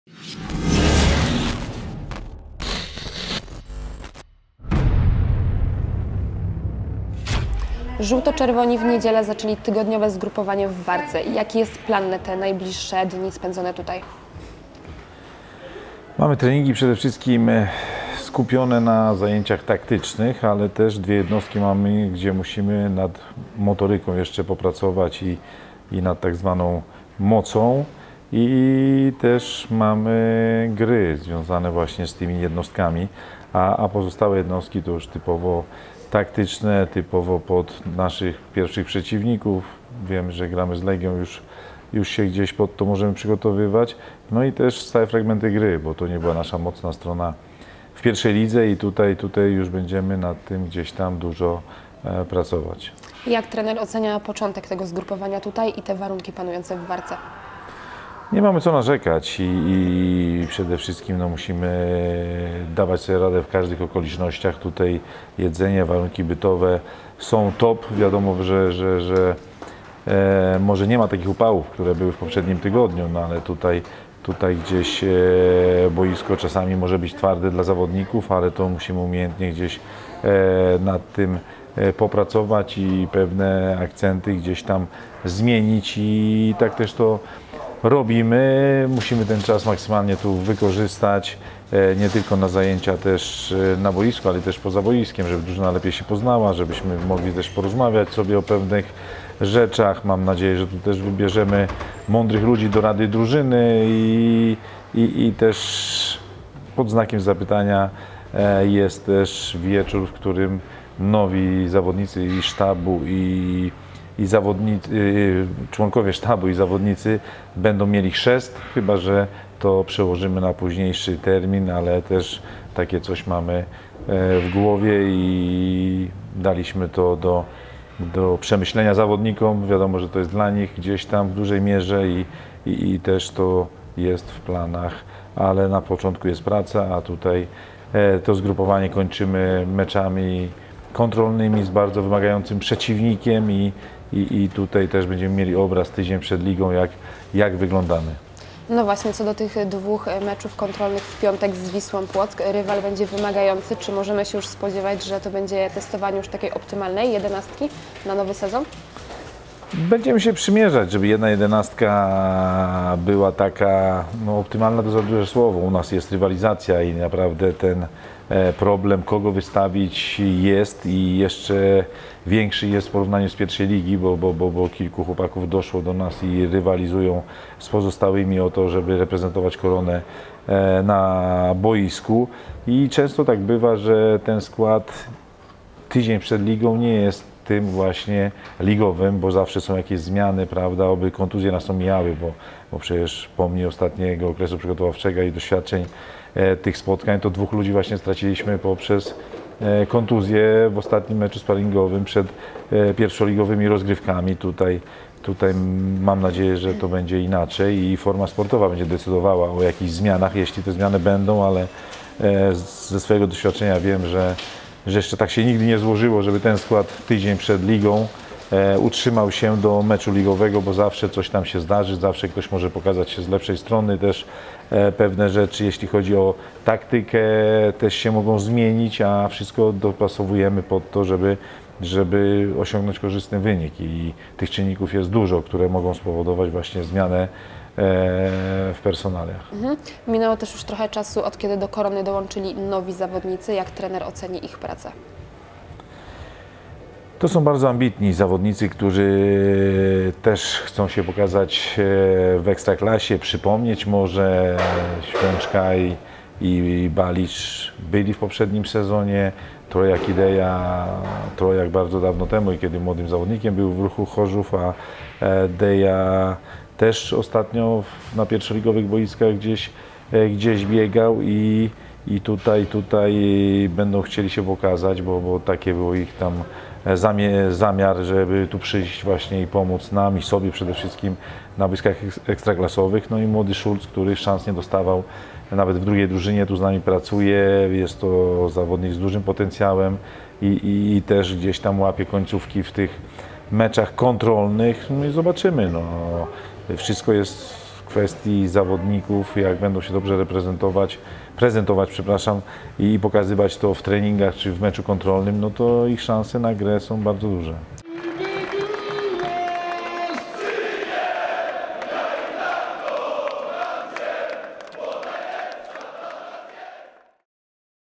Trener Ojrzyński o zgrupowaniu
Rozmowa z trenerem Leszkiem Ojrzyńskim na początek zgrupowania Korony Kielce w Warce.